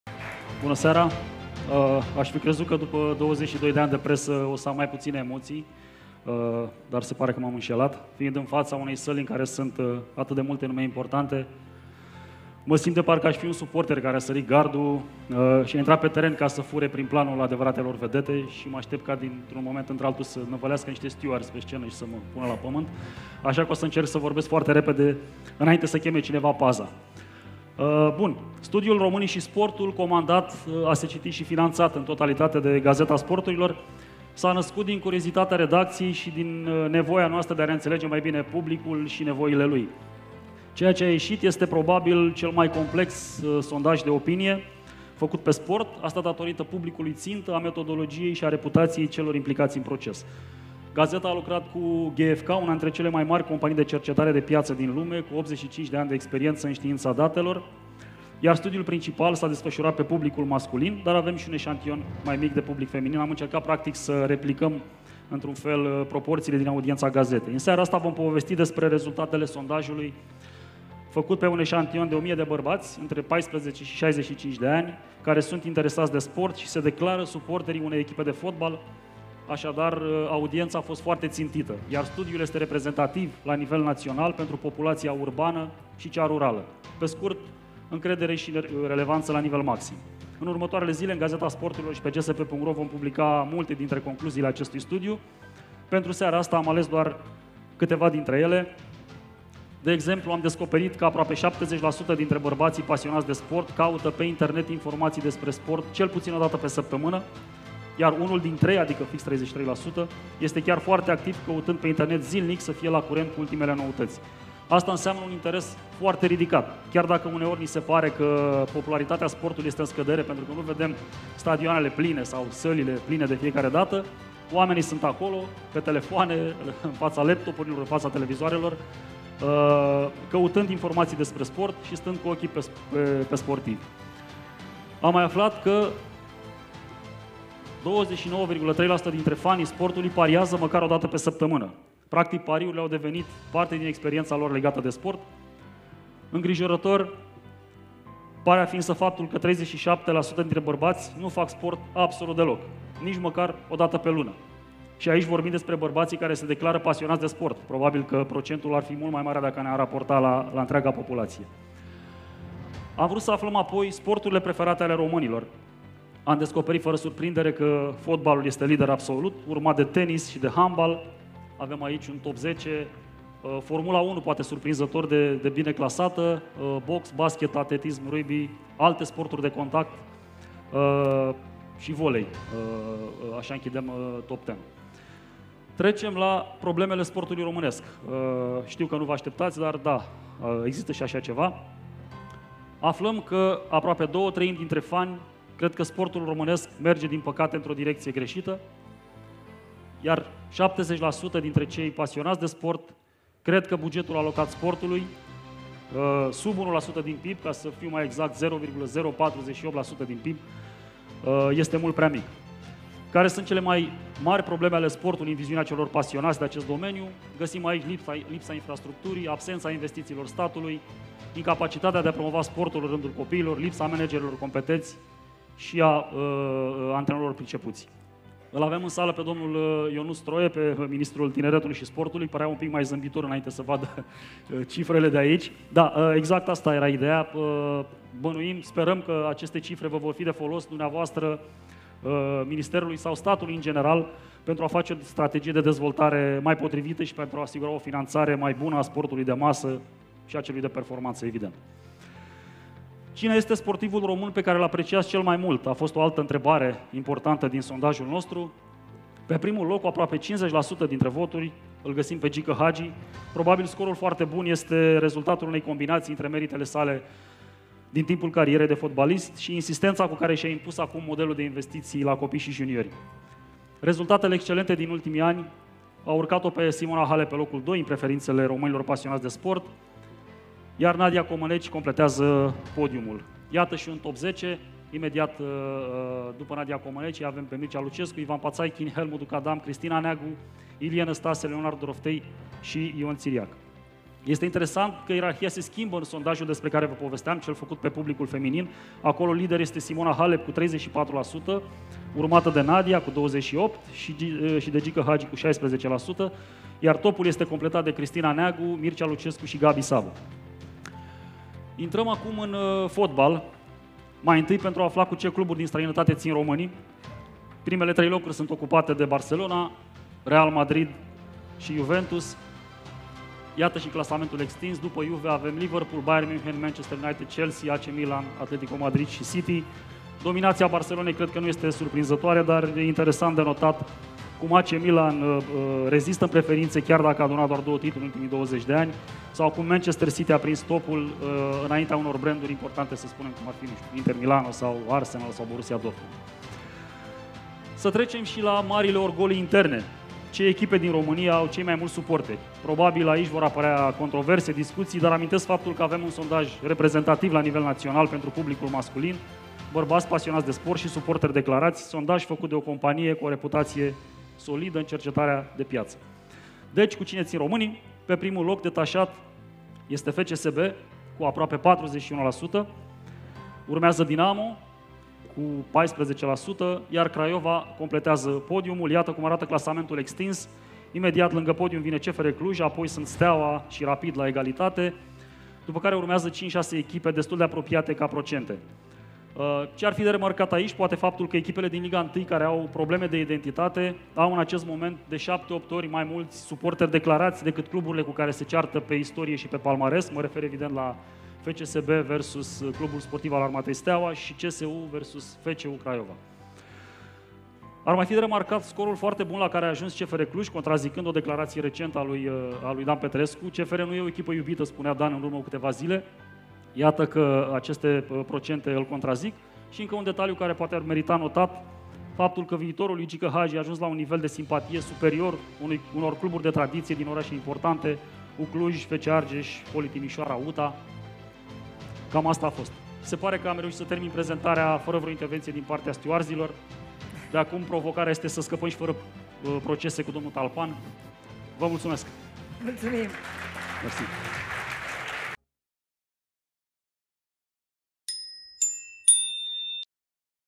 SONDAJ NAȚIONAL: FCSB are cei mai mulți suporteri! Gică Hagi e cel mai apreciat om din sport
FCSB este echipa din România cu cei mai mulți suporteri, arată un sondaj reprezentativ la nivel național comandat de Gazeta Sporturilor și realizat de GfK, una dintre cele mai mari companii de cercetare de piață din lume. REZULTATEL COMPLETE SUNT AICI

Gazeta a lucrat cu GfK, una dintre cele mai mari companii de cercetare de piață din lume, cu 85 de ani de experiență în știința datelor în peste 100 de țări, printre care și România. Sondajul comandat și finanțat de GSP s-a desfășurat în perioada octombrie - noiembrie, pe un public țintă format din bărbați pasionați de sport și suporteri declarați.